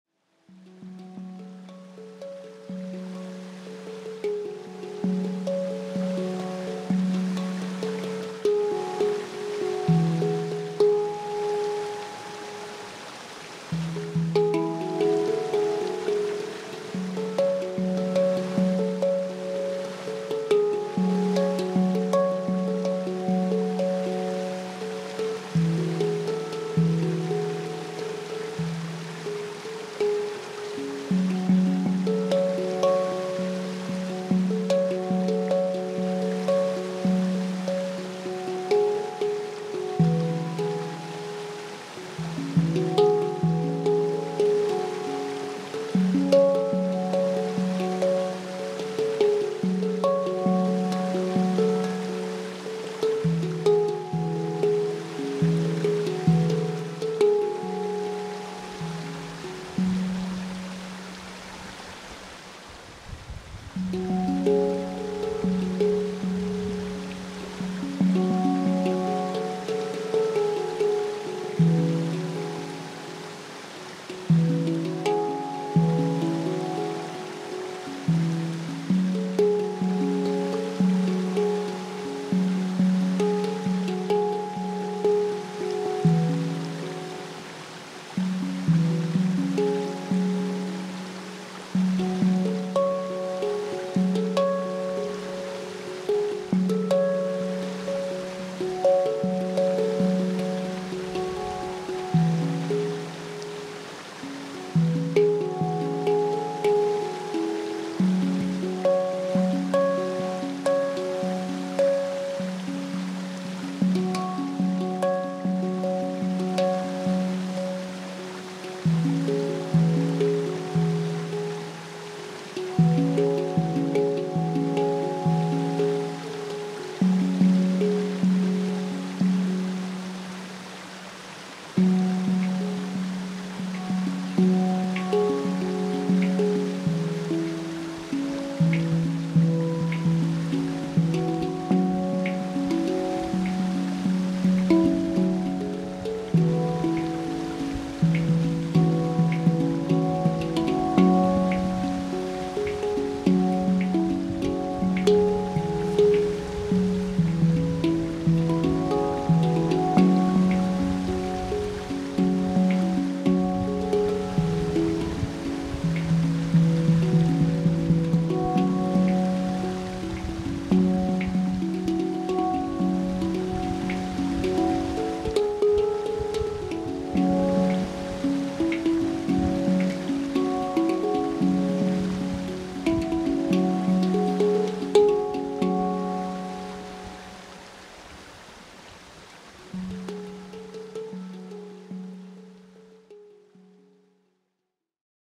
Soothing Handpan Meditation Music in Rainforest River | Nature Healing for Stress & Anxiety Relief
Take a moment with me 🍃 while I play you a tune 🎶
Welcome to my handpan meditation music video set in the heart of the rainforest in South East Queensland! Let the enchanting sounds of the handpan meld with the gentle flow of the river, as nature's symphony transports you to a state of deep relaxation, mindfulness, and mental well-being.

This unique meditation experience is perfect for those seeking relief from stress, anxiety, and depression. Immerse yourself in the soothing power of nature, as the handpan's melodic vibrations harmonize with the lush rainforest backdrop, creating an ambiance of tranquillity and peace.

Our handpan meditation music is specifically designed to foster mindfulness, promote mental health, and guide you on a journey to inner calm. Whether you're an experienced meditator or just beginning your journey, this beautiful nature escape will provide the perfect backdrop to help you unwind, recharge, and find balance in your life.

Experience the transformative power of mindful living and improve your mental health today!